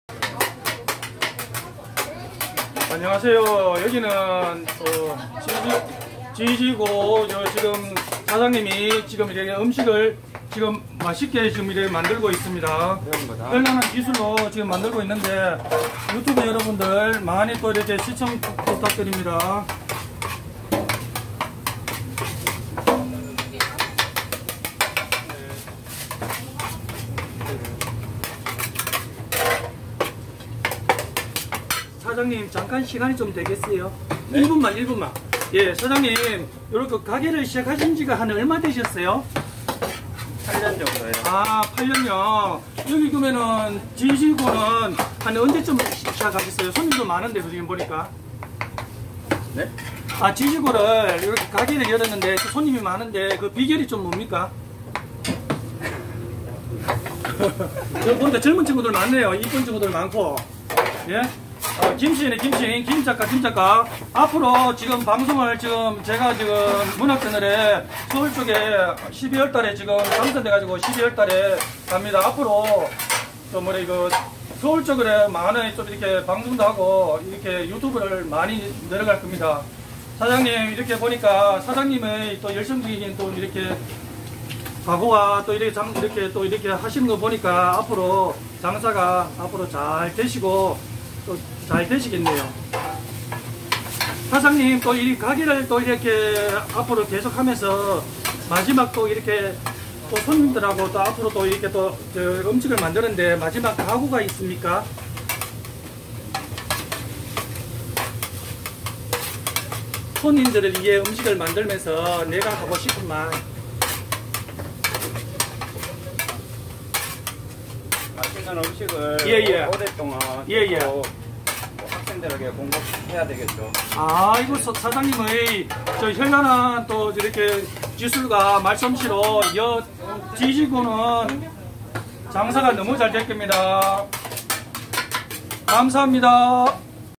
구미역옆 지지고 컵밥 ~~♡♡ 맛은 기본 사장님의 능숙한 솜씨 더 일품
사장님의 멋진 솜씨~~